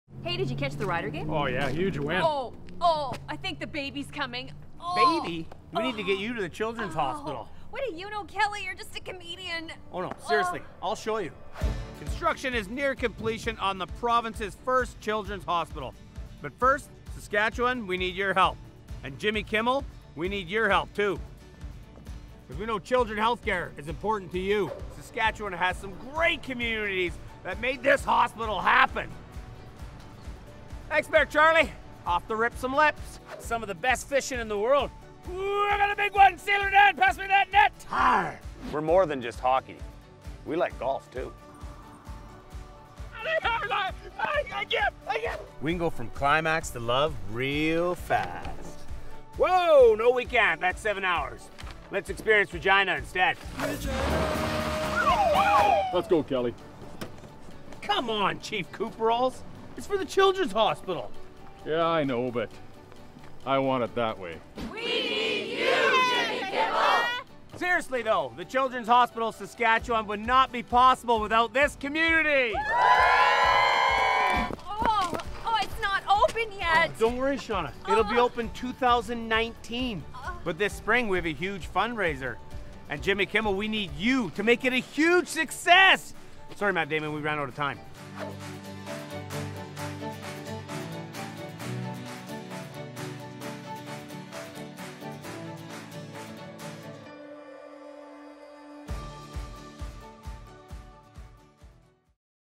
Care for Kids by Wiegers - The Kimmel Campaign!
Care for Kids by Wiegers REALLY wants Jimmy Kimmel to join us at our 2019 fundraising event! We've set a fundraising goal of at least $400,000 but with his support, we could far exceed our goal and better help the children of Saskatchewan. Please watch, share, and ALWAYS include KimmelCares. Please help us get Jimmy Kimmel's attention and support!